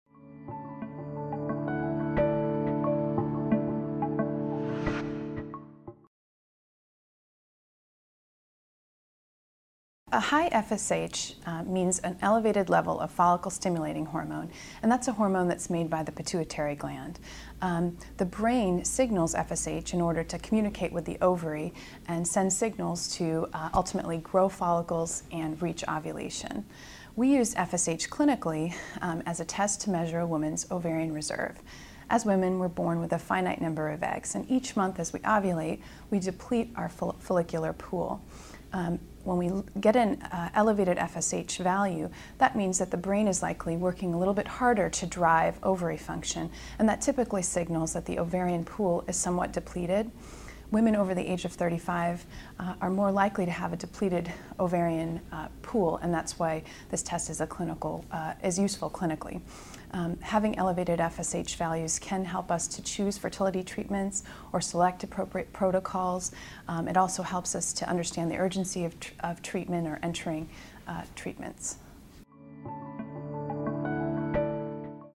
What Is High FSH?
Dr. Pavna Brahma, a fertility doctor with Reproductive Biology Associates in Atlanta, explains that FSH - follicle stimulating hormone - is a good measure of ovarian reserve. With high FSH, the ovarian reserve is depleted. By measuring FSH, your fertility doctor has a better understanding of what fertility treatment or protocol will be best for you.